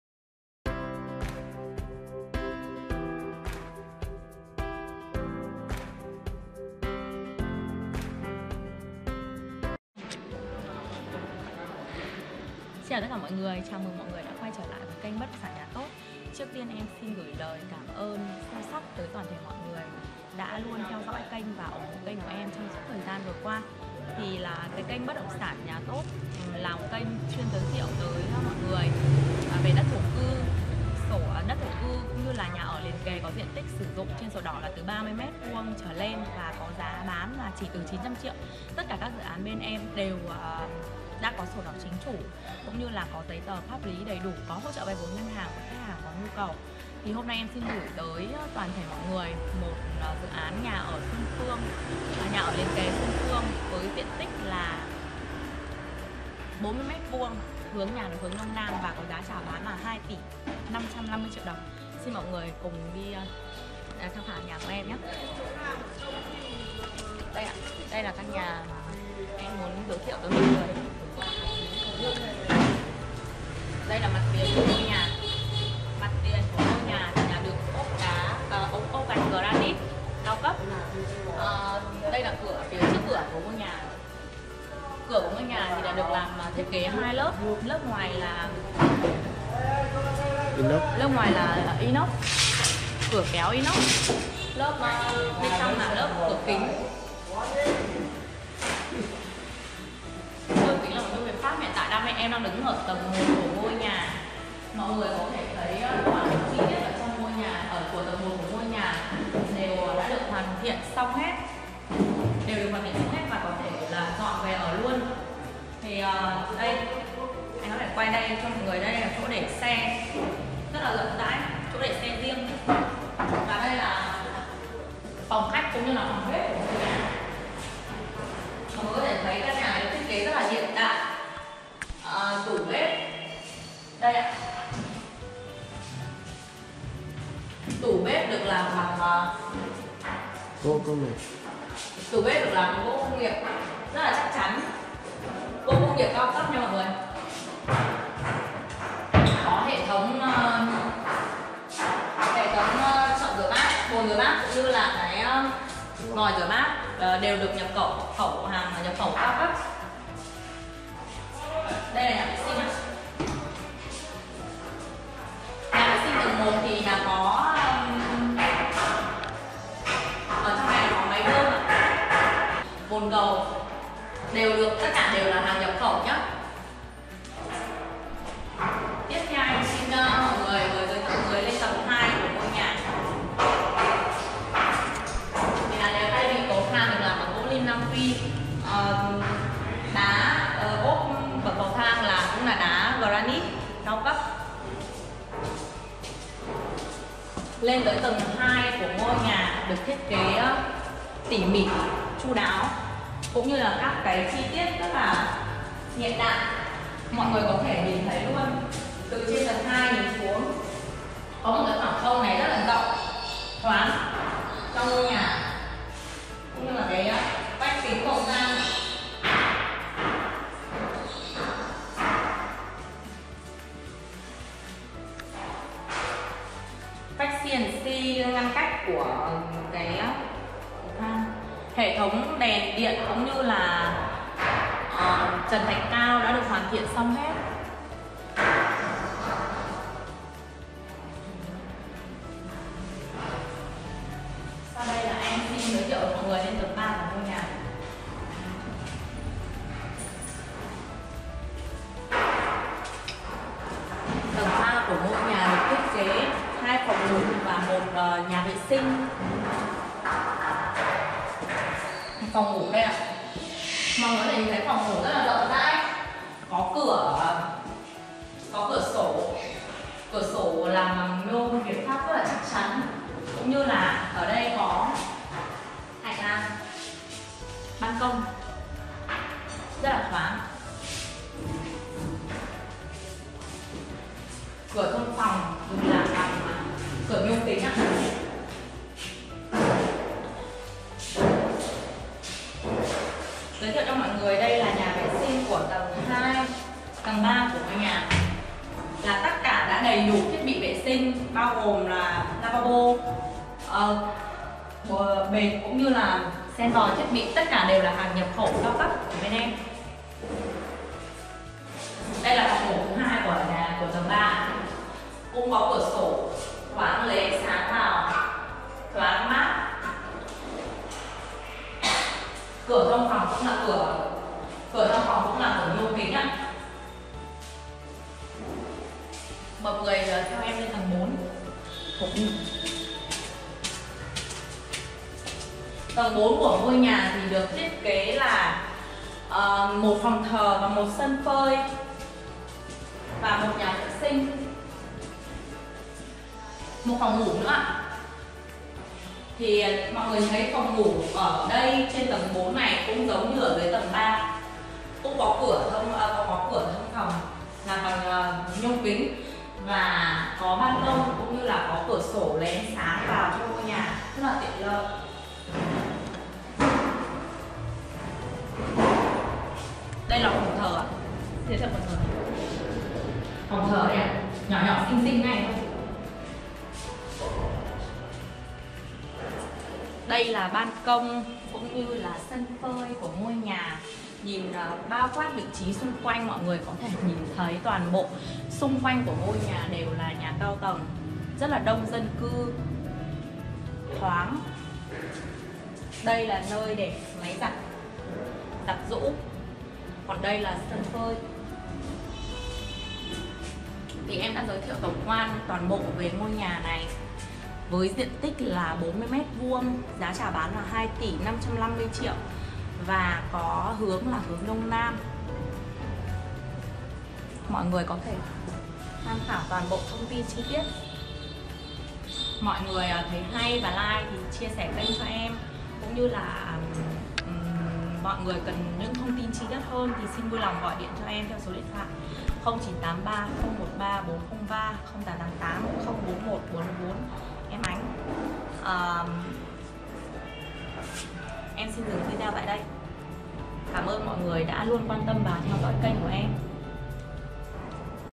Bán nhà 4 tầng 40m2 giá rẻ nhất phường Xuân Phương, quận Nam Từ Liêm, Hà Nội | nhà TỐT
nhà TỐT | Bán nhà 4 tầng 40m2 giá rẻ nhất phường Xuân Phương, quận Nam Từ Liêm, Hà Nội
- Diện tích: 40m2
- Hướng nhà: Đông Nam
- Giá bán: 2.55 Tỷ
- Thiết kế: 4 tầng, 3PN, 3WC, phòng khách, phòng bếp, phòng thờ
- Bán nhà sổ đỏ chính chủ.
© Bản quyền thuộc về BÁN NHÀ HÀ NỘI
© Copyright by BÁN NHÀ HÀ NỘI ☞ Do not Reup